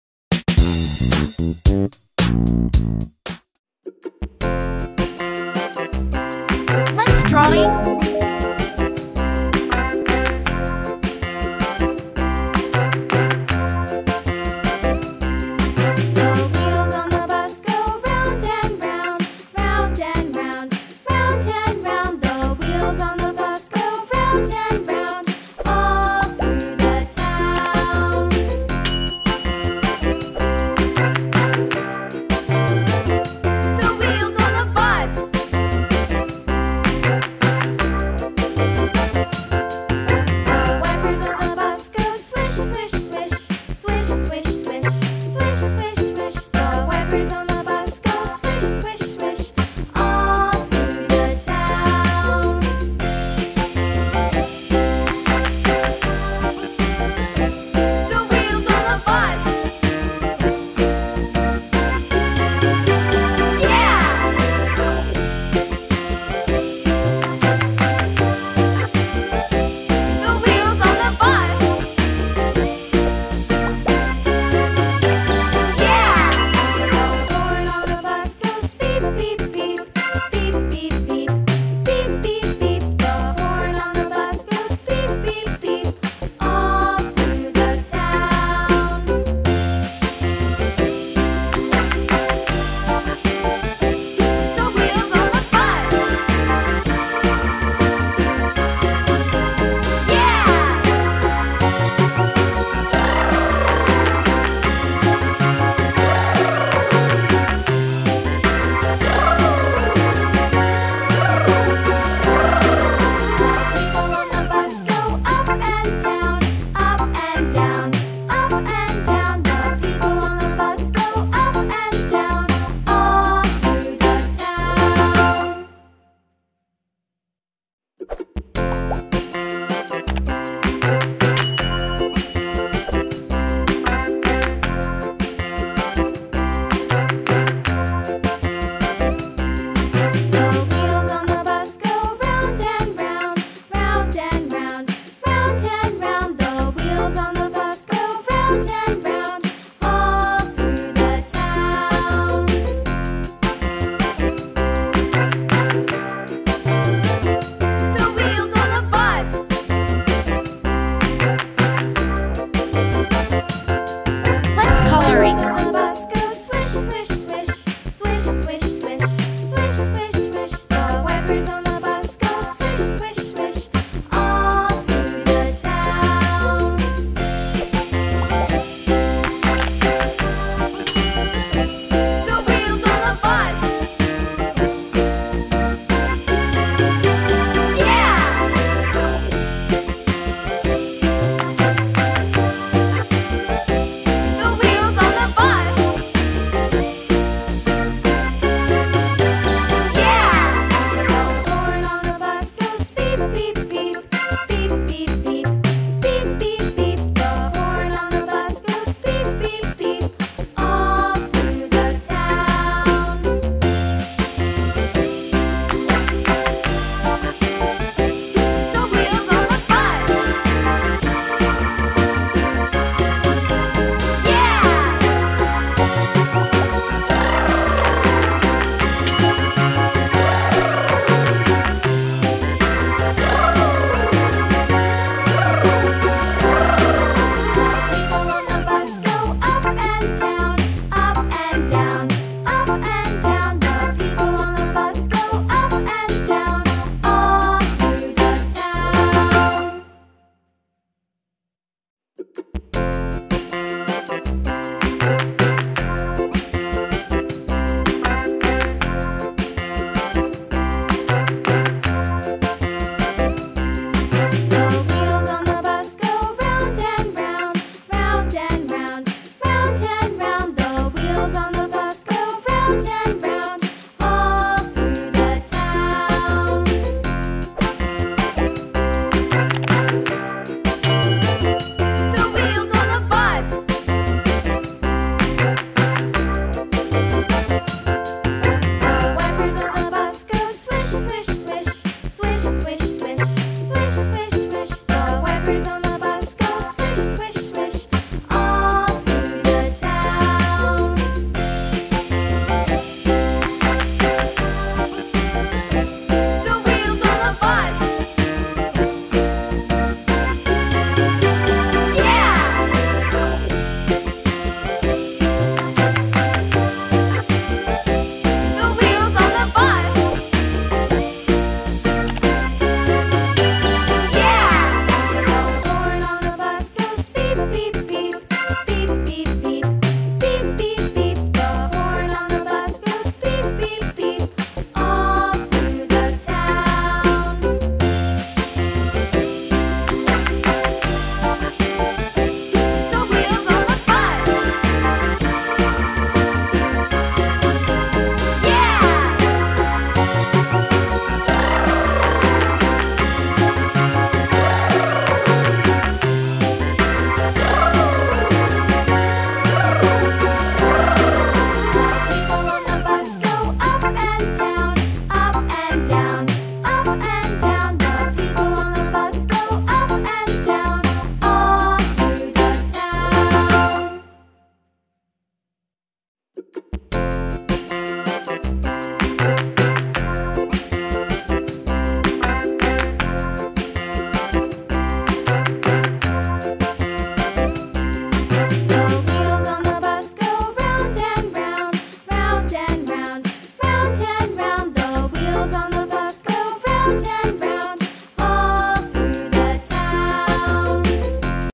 Drawing and coloring larva | Coloring Pages For Kids | Children's Creative Media #14
Hello friends 💖

Today I draw and color larva using watercolor very easily

Happy watching friends